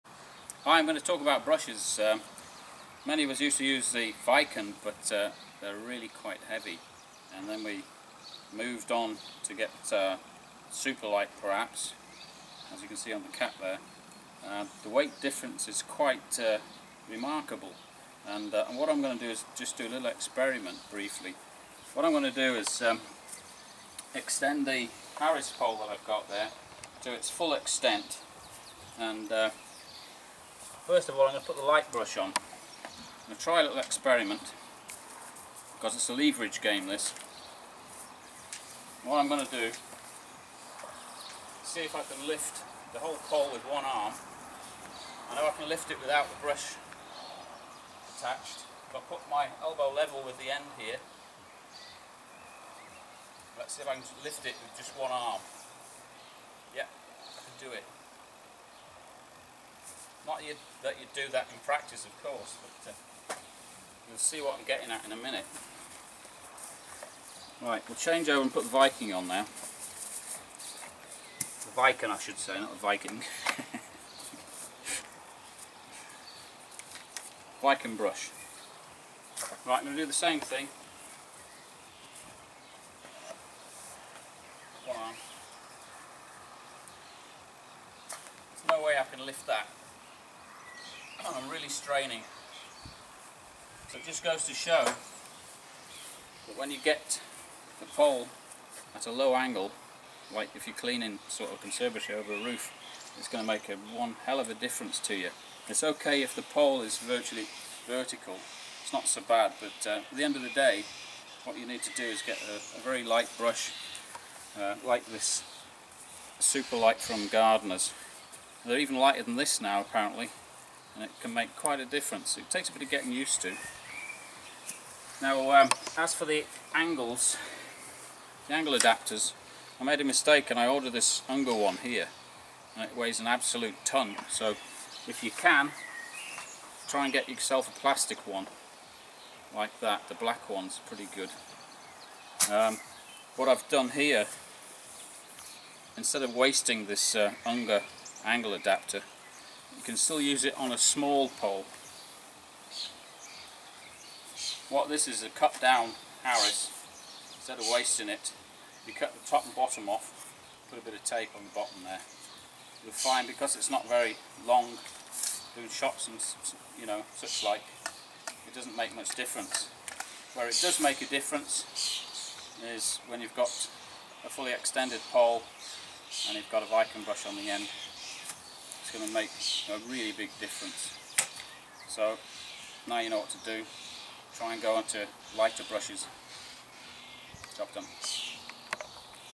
Window cleaning Tips - Water Fed Pole Brush Weight Test
Window Cleaning Tips, - Water Fed Pole Brush. Here I test the weight of two different brushes. Choosing an ultra light brush can transform your window cleaning experience. Also check out your angle adapter, since this can be a big weight factor also.